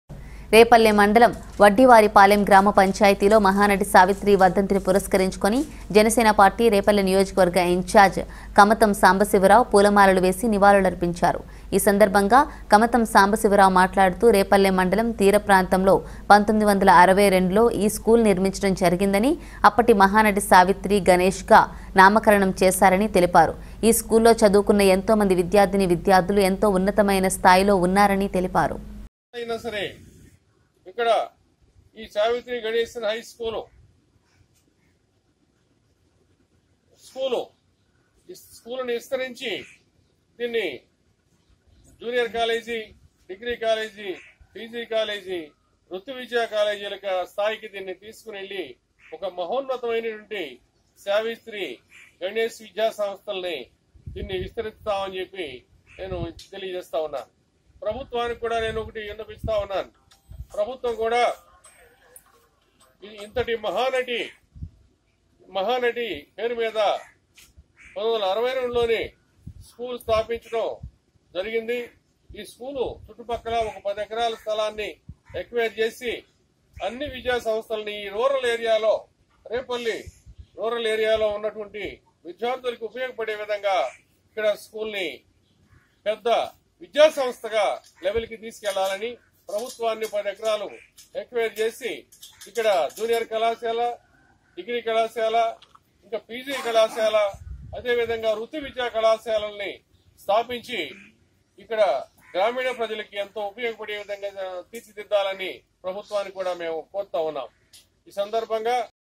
రేపల్లె మండలం వడ్డివారిపాలెం గ్రామ పంచాయతీలో మహానటి సావిత్రి వర్ధంతిని పుర్సకరించిన జనసేన పార్టీ
రేపల్లె మండలం వడ్డివారిపాలెం గ్రామ పంచాయతీలో మహానటి సావిత్రి వర్ధంతిని పురస్కరించుకొని జనసేన పార్టీ రేపల్లె నియోజకవర్గ ఇన్చార్జ్ కమతం సాంబశివరావు  పూలమాలలు వేసి నివాళులర్పించారు. ఈ సంద‌ర్భంగా  కమతం సాంబశివరావు మాట్లాడుతూ రేపల్లె మండలం తీర ప్రాంతంలో 1962లో ఈ స్కూల్ నిర్మించడం జరిగిందని,  అప్పటి మహానటి సావిత్రి గణేష్ గా నామకరణం చేశారని తెలిపారు. ఈ స్కూల్లో చదువుకున్న ఎంతో మంది విద్యార్థినీ విద్యార్థులు  ఎంతో ఉన్నతమైన స్థాయిలో ఉన్నార‌ని తెలిపారు.